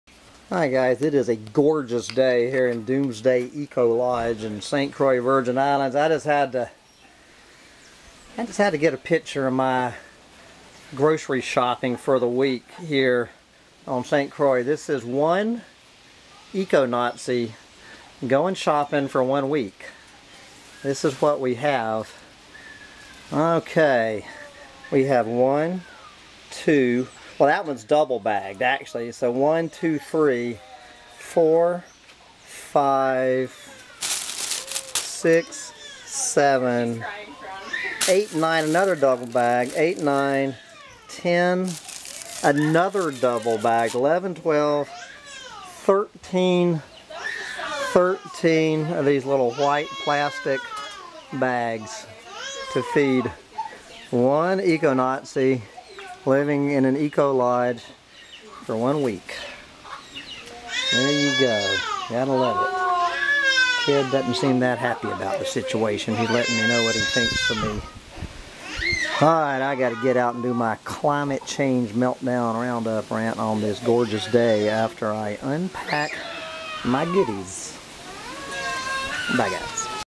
An Eco-Nazi Goes Grocery Shopping in the End Times in Paradise
Here is what it looked like in the eco-lodge kitchen when I returned from the grocery store with a total of 19 items.